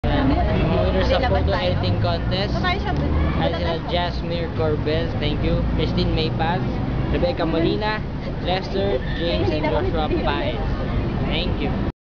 Joshua Dionisio: Photo Editing Contest!!!
Joshua Dionisio is greeting the five (5) winners of the Photo Editing Contest of Joshua Addicts Family!

Pls. LIKE the OFFICIAL FAN PAGE of Mr. Joshua Dionisio!!!

Thanks!